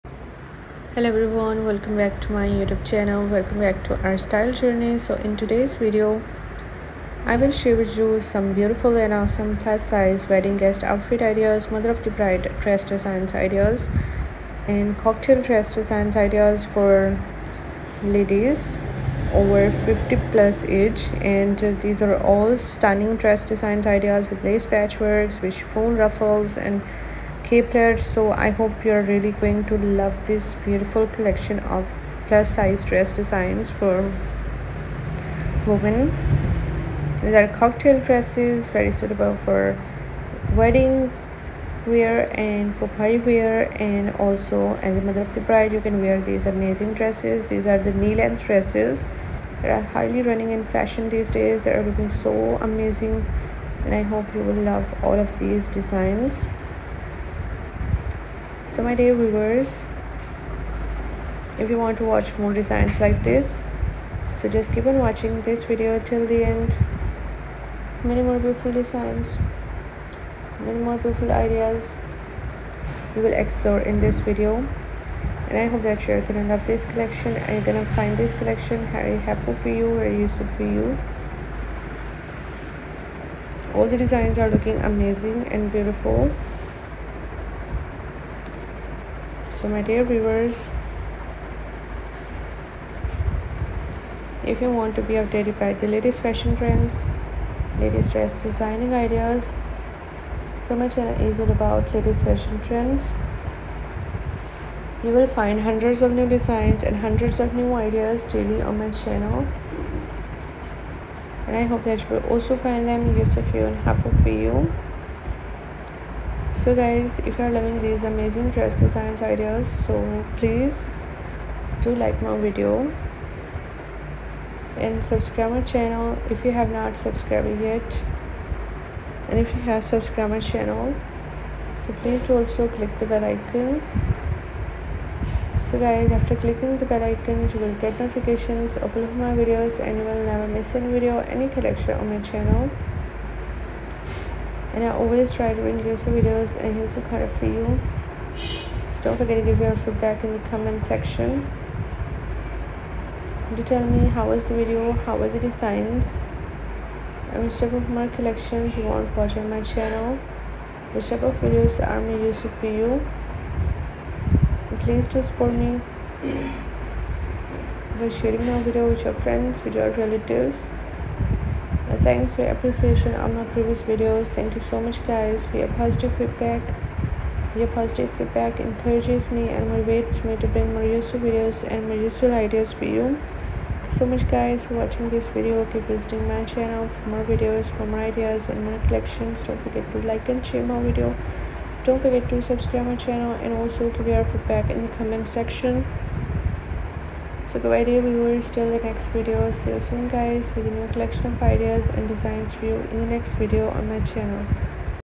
45+ Unique plus size mother of the bride outfit ideas\\wedding guest dress designs
Elevate your style for every wedding celebration with our "Wedding Guest Outfit Ideas" showcase! 👗💐 In this inspiring video, we unveil a curated collection of chic and elegant ensembles meticulously crafted to ensure you're the best-dressed guest at any wedding affair.

🌟 Explore a range of outfit options suitable for various wedding themes and venues – from classic cocktail dresses to modern jumpsuits and sophisticated two-piece sets, each ensemble is designed to make a statement while respecting wedding etiquette.

💃 Immerse yourself in the details of each look, where accessories, hairstyles, and makeup come together to complete the perfect wedding guest ensemble. These outfit ideas aren't just about dressing up; they're a celebration of love, joy, and the beauty of being part of someone's special day.